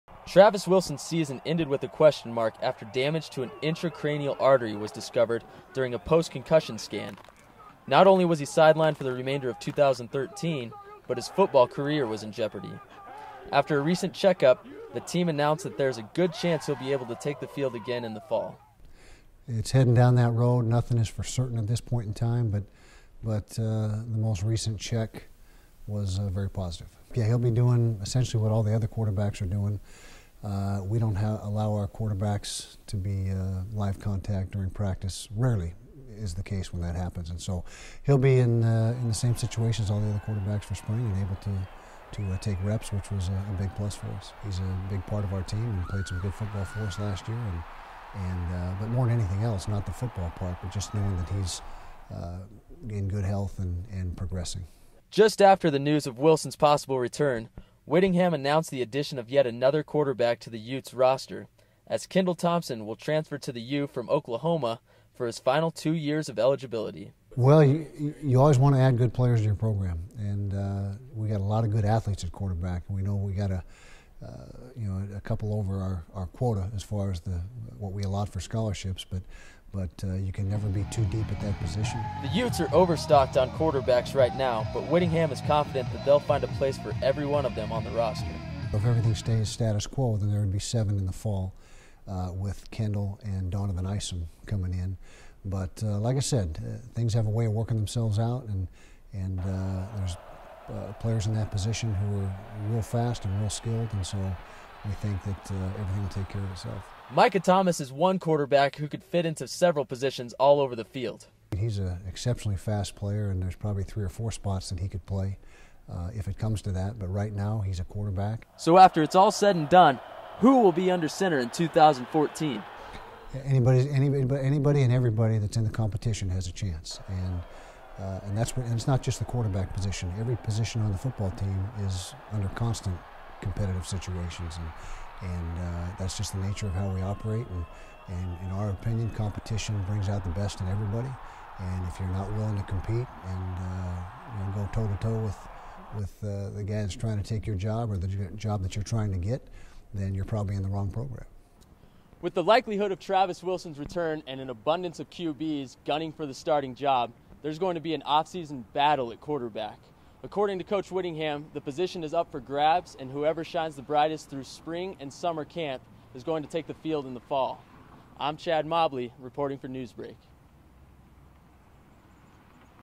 Talking 2014 Utah QB's with Kyle Whittingham
One on one interview with Utah head coach Kyle Whittingham to talk about the abundance of quarterbacks heading into spring football for the Utes